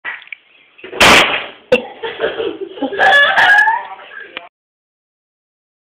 Very Painful Baby Faceplant
my nephew faceplants xD uhh. the video is very old, so poor cellphone filming xD bad quality